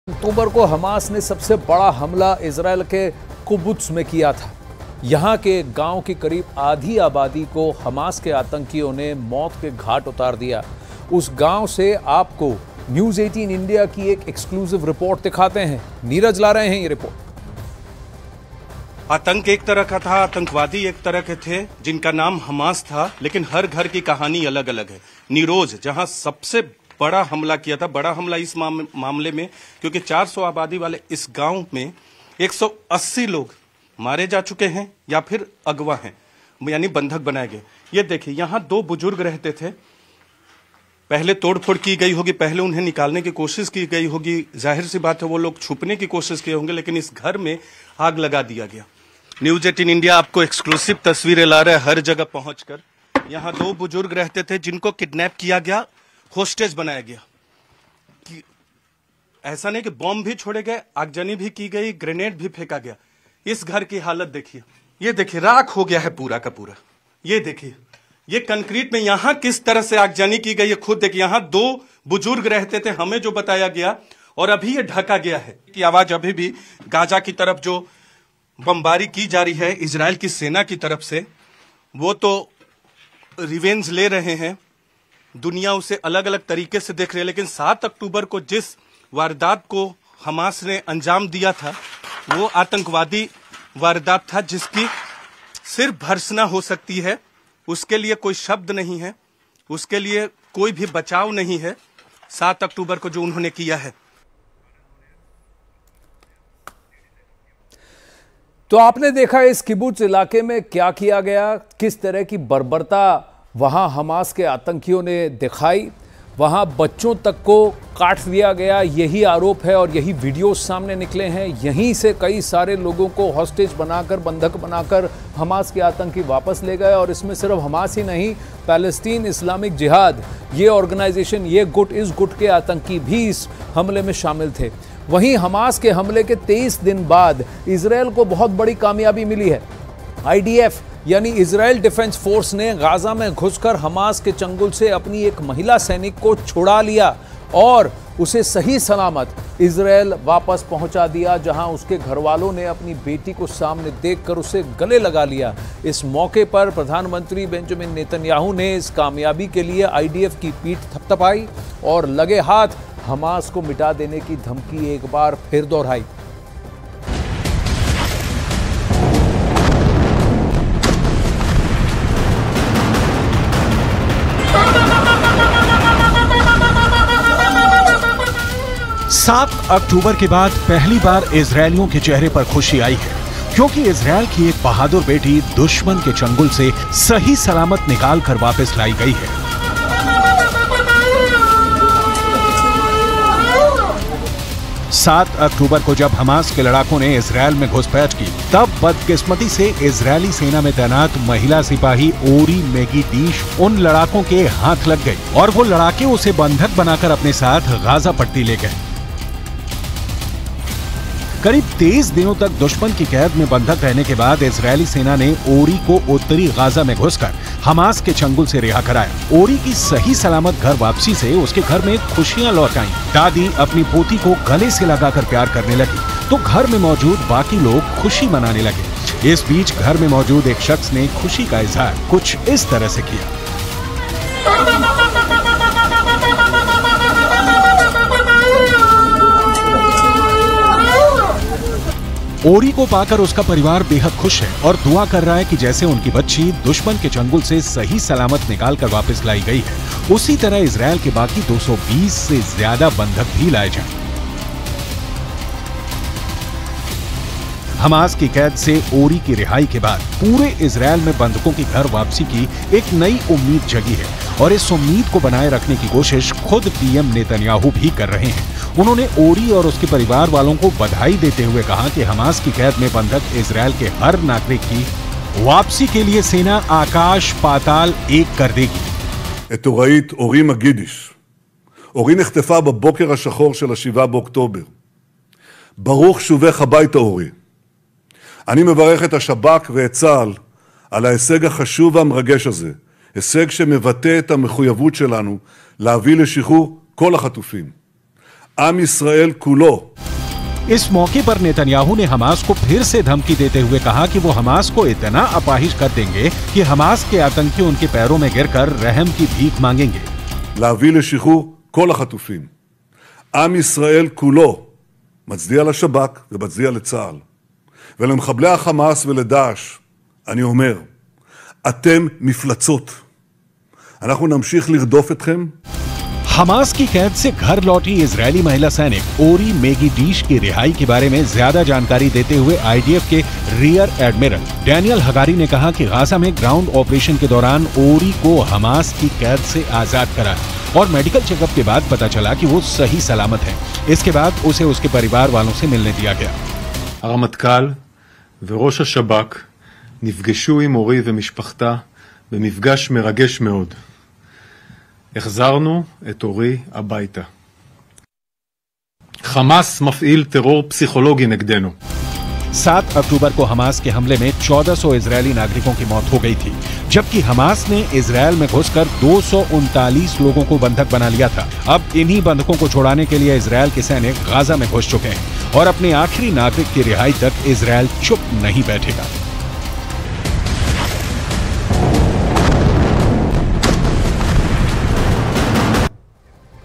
Israel Hamas War: Hezbollah ने मचाई तबाही! | Palestine War | N18V | World War | Breaking News
Israel Hamas war: On October 7, Hamas carried out its biggest attack in a Qubbutz in Israel. Nearly half the population of a village here was killed by Hamas terrorists, we show you NEWS18 India's exclusive report from that village.
Israel Hamas war : 7 अक्टूबर को हमास ने सबसे बड़ा हमला इजराइल के कुबुत्ज में किया था। यहां के एक गांव की करीब आधी आबादी को हमास आतंकियों ने मौत के घाट उतार दिया, उस गांव से आपको NEWS18 इंडिया की एक्सक्लूसिव रिपोर्ट दिखाते हैं.